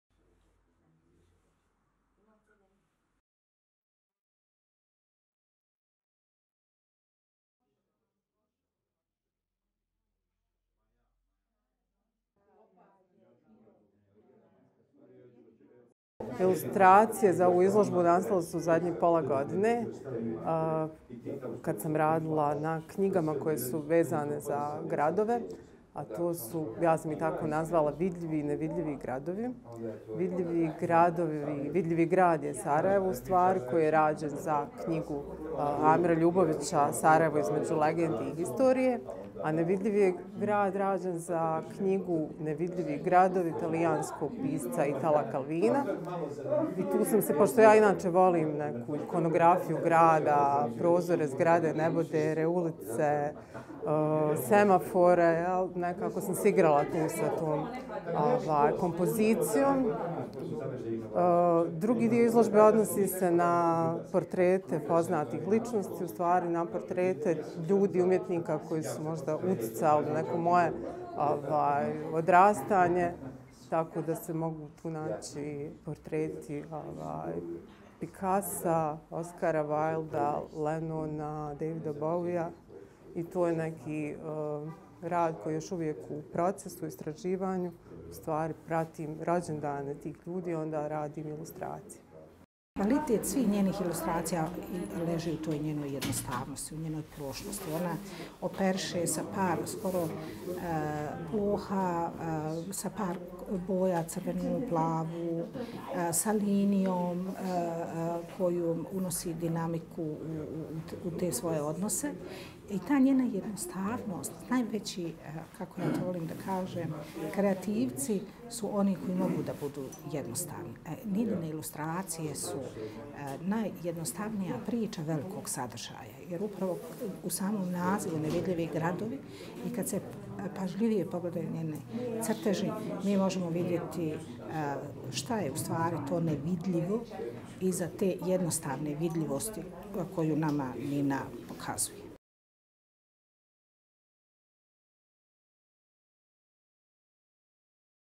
Nevidljivi gradovi i portreti na izložbi radova Aleksandre Nine Knežević
SARAJEVO, 16. februara (FENA) - Izložba radova Aleksandre Nine Knežević nazvana "Nevidljiva izložba" otvorena je u okviru projekta "Umjetnost u izlogu" u sarajevskoj Galeriji Udruženja likovnih umjetnika primijenjenih umjetnosti BiH (ULUPUBiH).